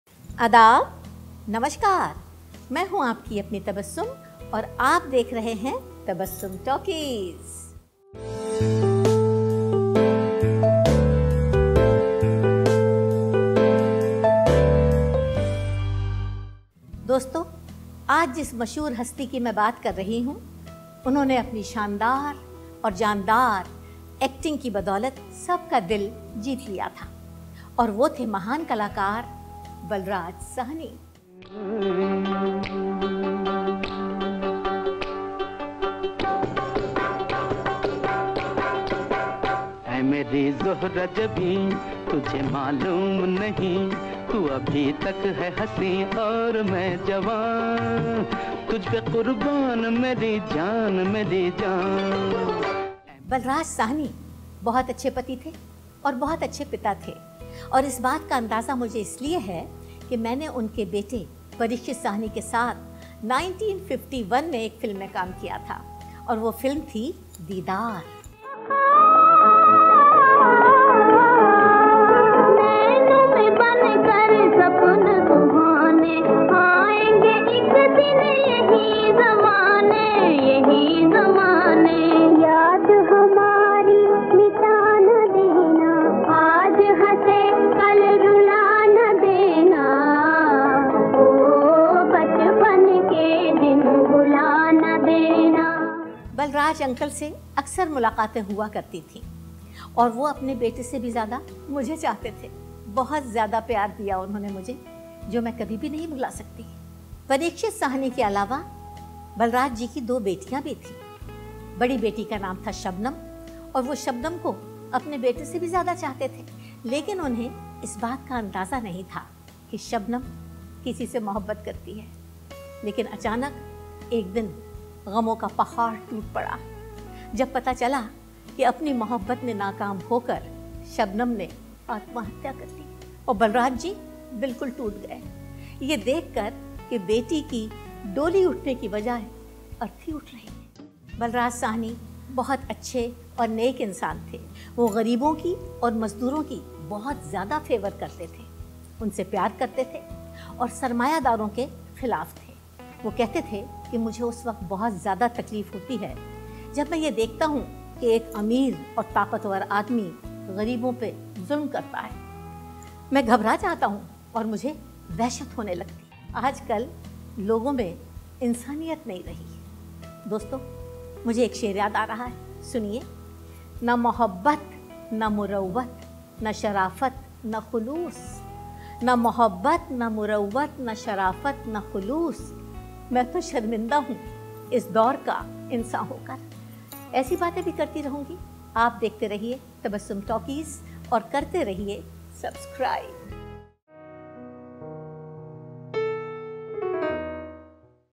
An Inside Look Into Balraj Sahni's Life | Tabassum Talkies
In this episode Tabassum talks about how  wonderful a person Balraj Sahni was and shares a tragic story that left him heart broken